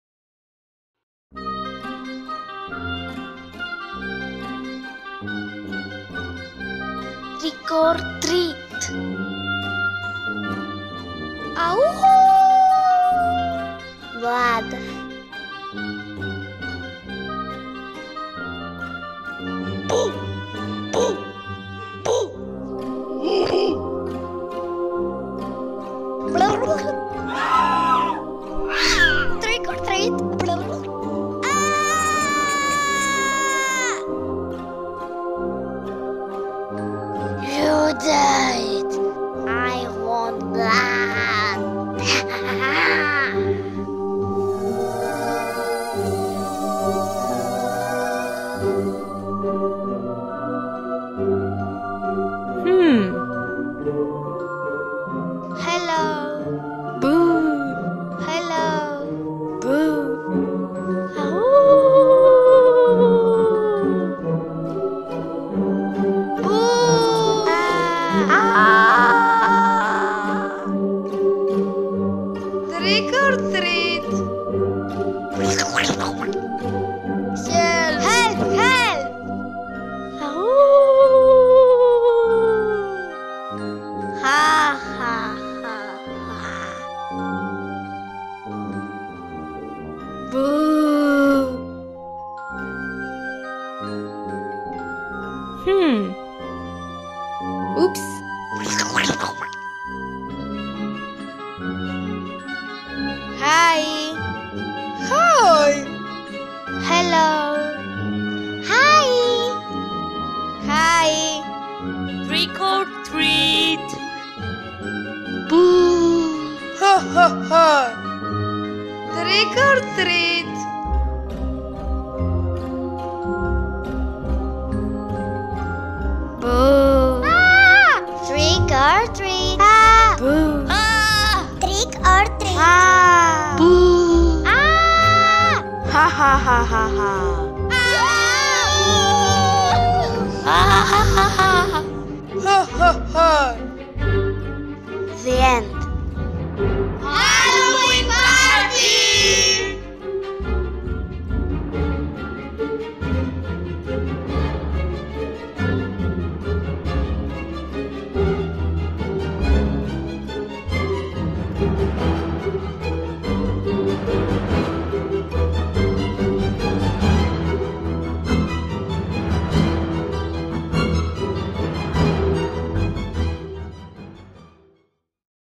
trick or treat
Η ταινία "Trick or Treat?" δημιουργήθηκε από παιδιά δημοτικού στα πλαίσια εργαστηρίου κινούμενης εικόνας στον όμιλο φροντιστηρίων Γνώση Χωρίς Σύνορα.

Σχεδιασμός και υλοποίηση εργαστηρίου: Μαργαρίτα Σιμοπούλου